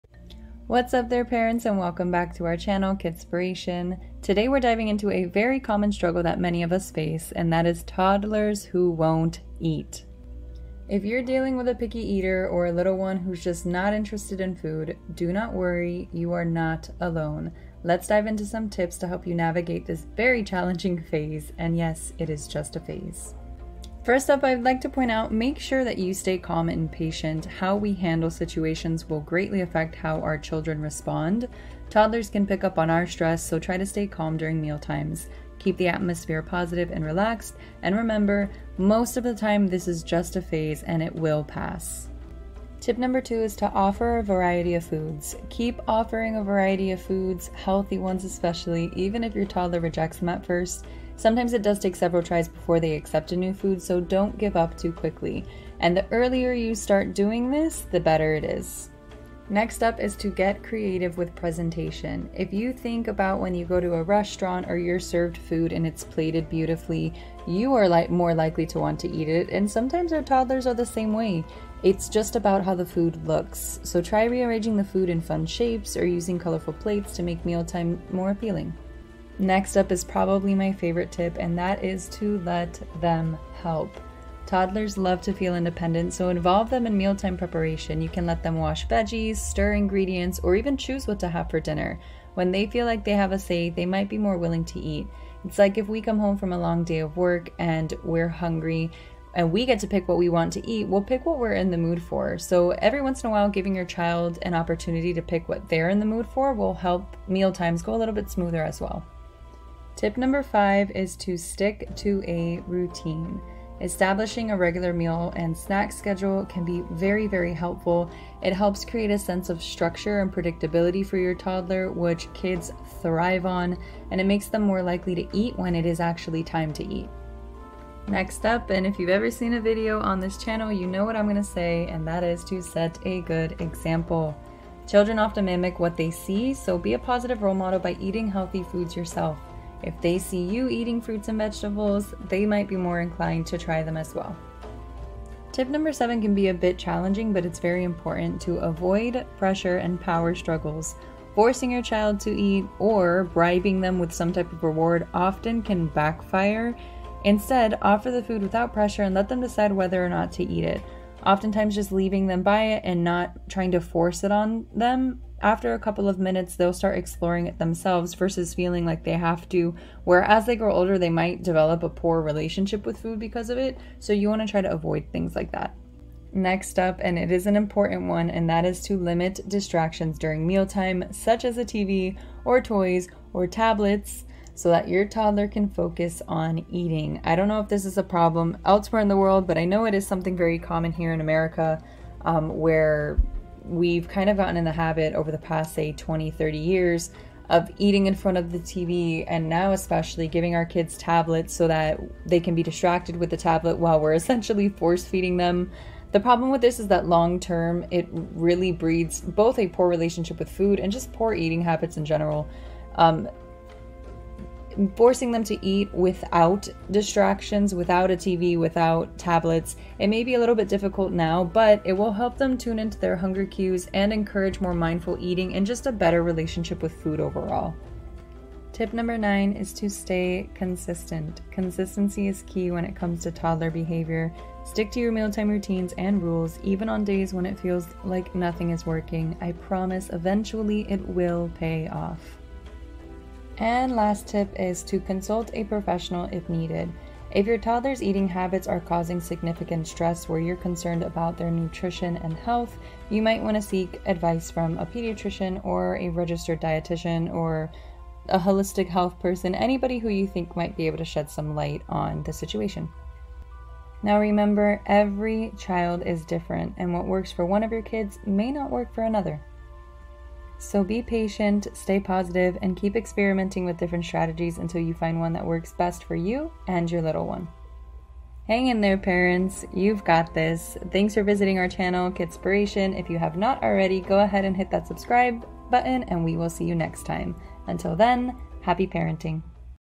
What To Do When Your Child Won't Eat : How To Handle Picky Eaters | Advice For Mealtime Struggle
Is your toddler not eating? Do your kids give you a hard time at meal time? Well this video is for you! Today we will address 10 parenting tips for helping your picky eaters be. well. NOT SO PICKY! Whether you have a toddler who won't eat, a young child who won't eat, or a teen who gives you a hard time, this video will give you plenty of ideas to help meal time go much smoother!